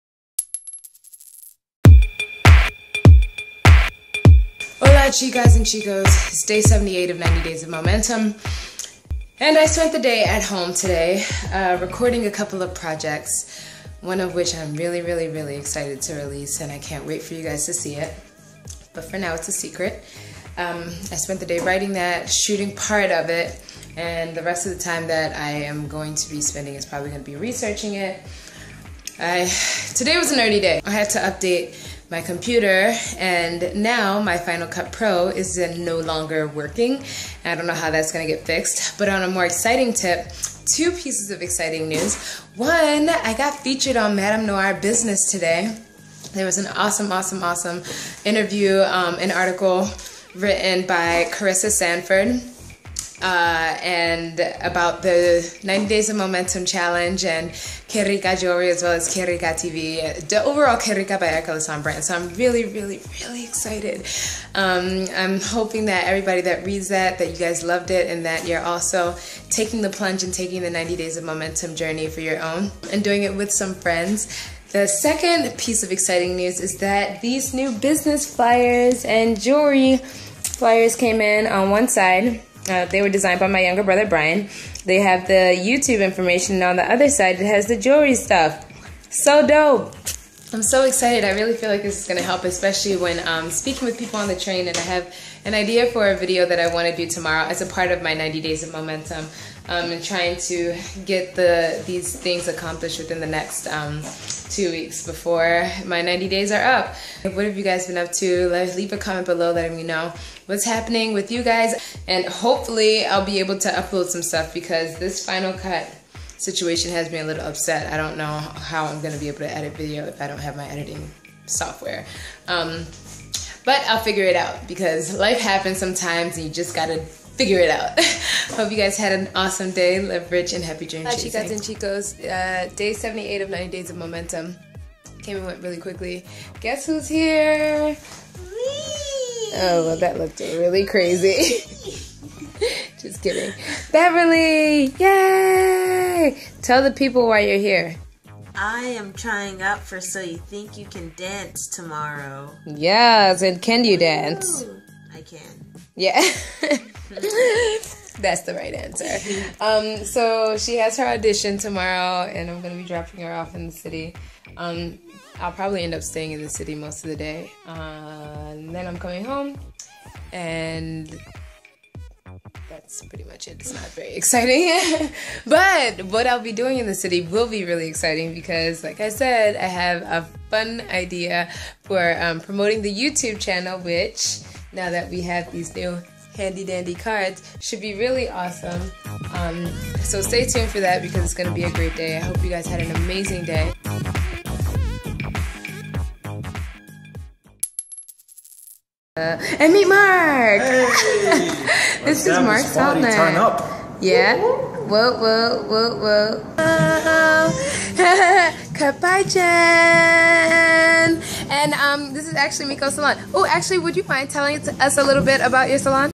90 Days of Momentum - Day 78
Day 78 of 90 Days of Momentum, Erica expresses her frustration over some computer issues.  But receives exciting news about the brand and Beverly, the youngest member of the Lasan clan, comes up to visit for the weekend!  :)